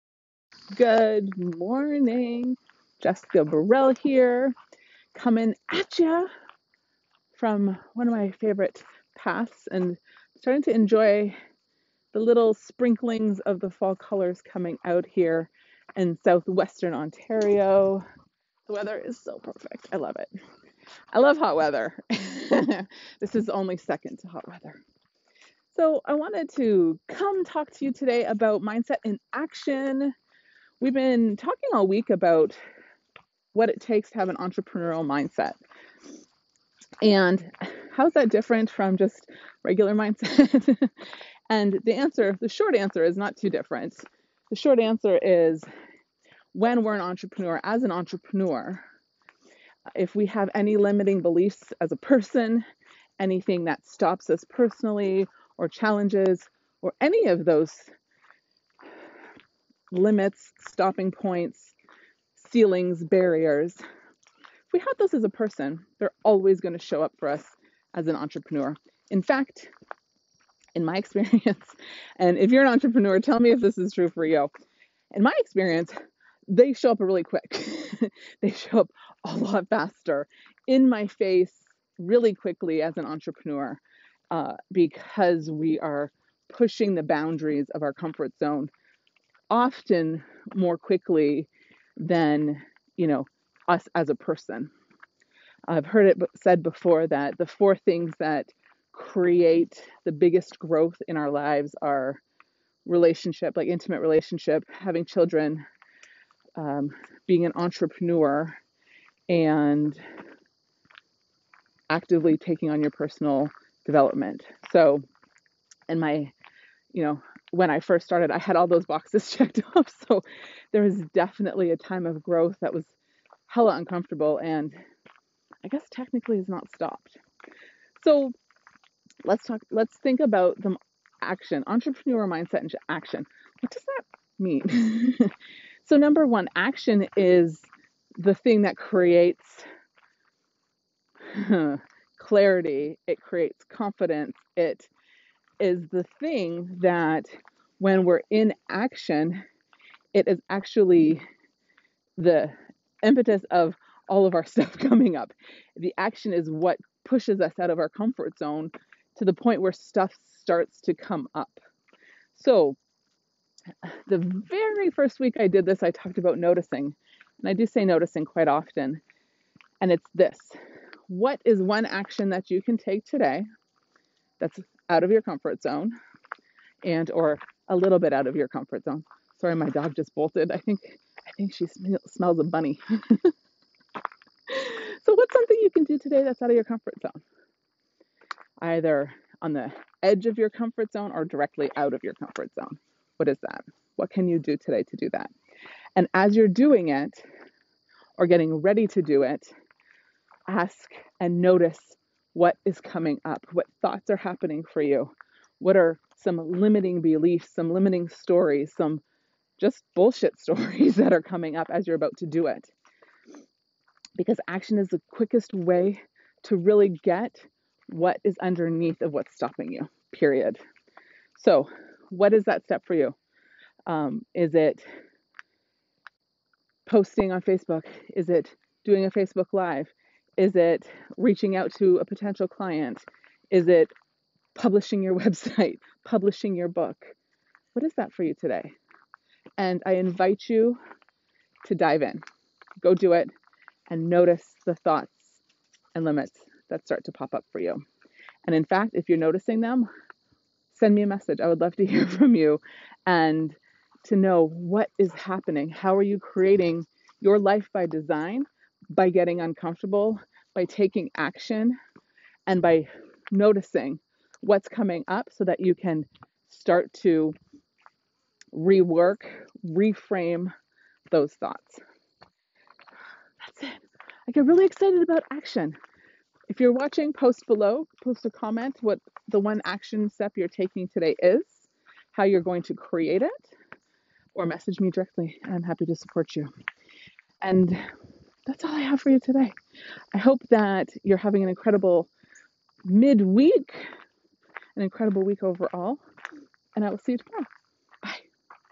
10 Minute Morning Mindset: Entrepreneur - Mindset in Action
Welcome back to 10 Minute Morning Mindset, we are continuing on with Entrepreneurial Mindset and today is all about taking action.